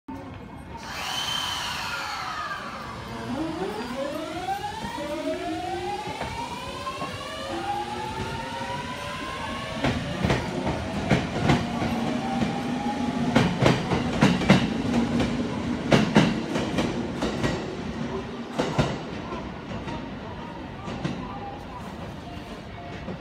【電車床下音シリーズ】JR中央総武線209系500番台 ブレーキ緩解＆VVVF音
電車床下音シリーズ！！
JR東日本209系500番台のブレーキ緩解音です^^